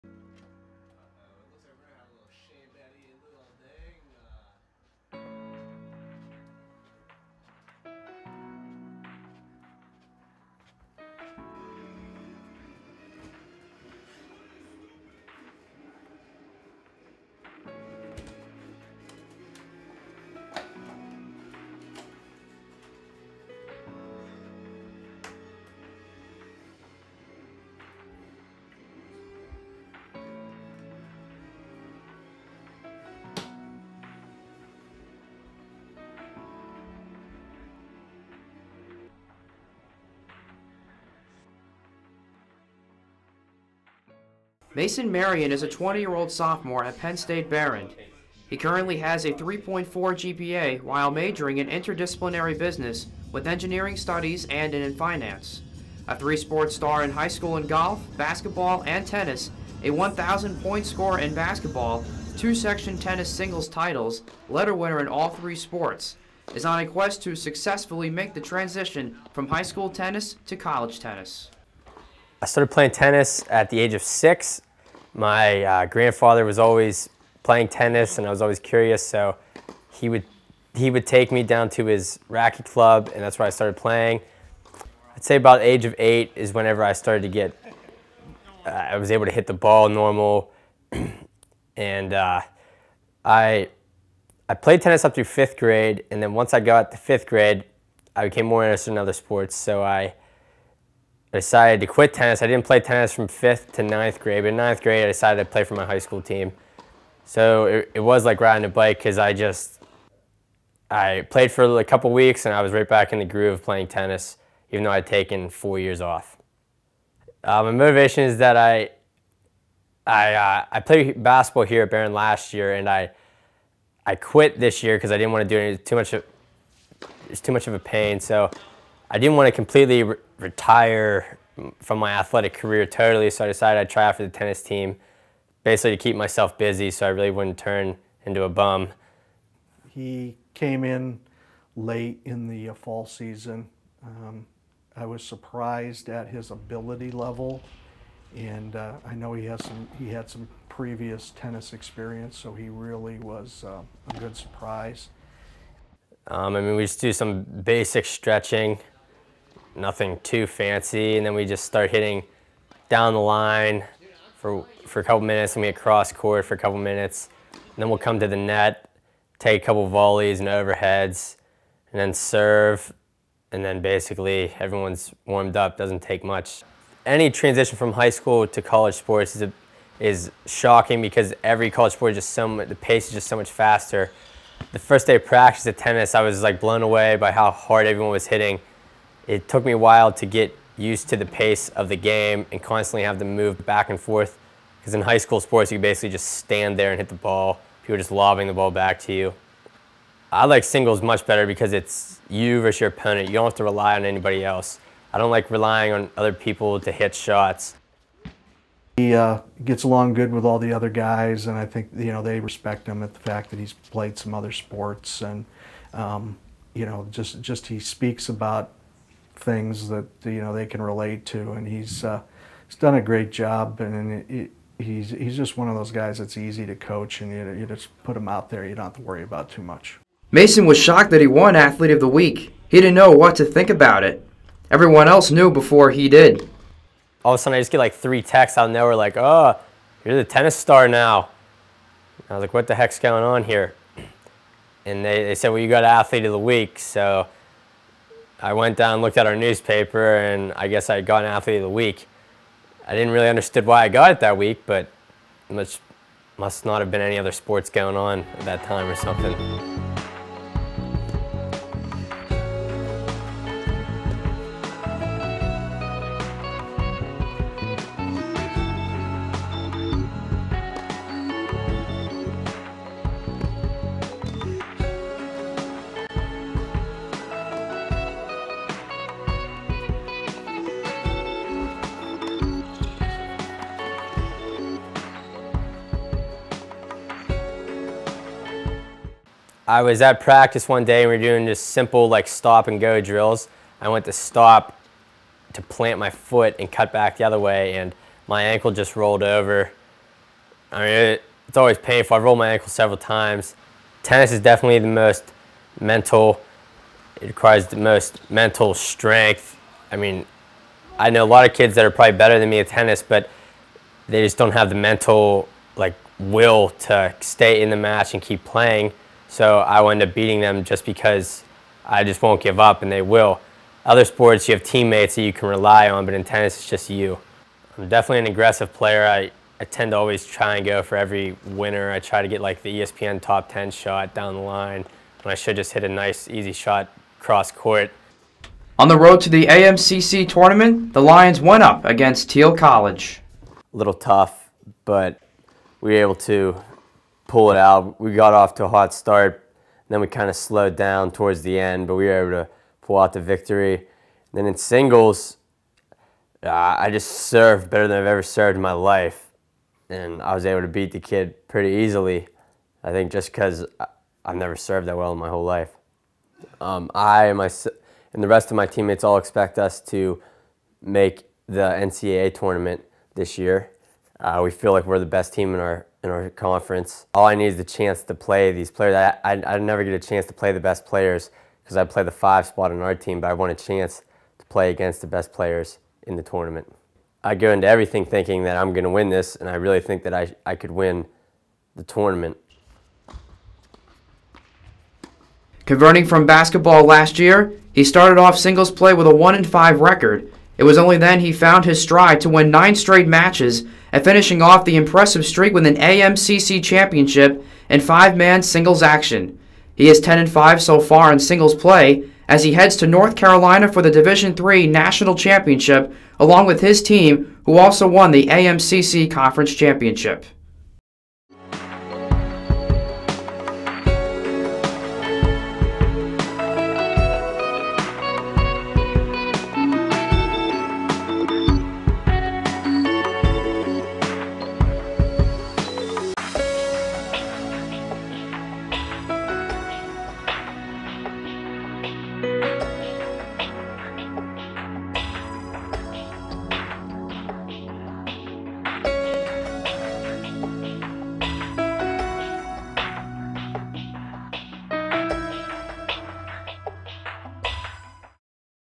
Tennis Documentary
This is a story about Mason Marion, the tennis player, and his pursuit of converting from high school tennis to college tennis and from college basketball last year to tennis this year.  I would like to thank relaxdaily for supplying the music.